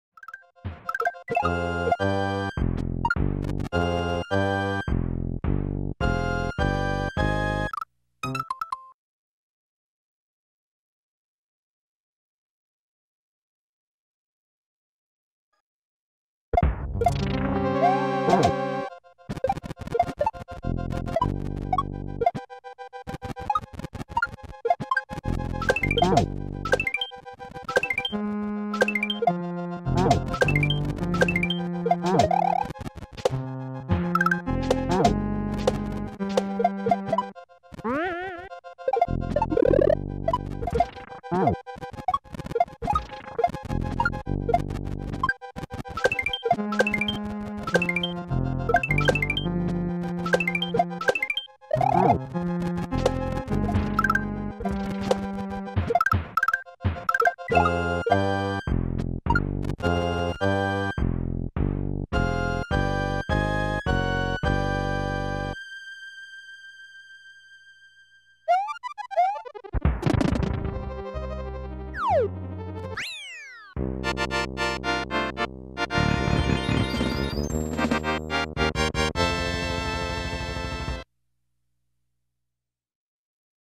teamPipe it's fine, ok
fun shell level using the lava a little bit, some parts are more precise than i wanted but oh well. I bonked my head at the end and messed up my strat for the fireballs, which is actually pretty easy, fortunately i saved it :D

ID: FF8B-0000-031B-A481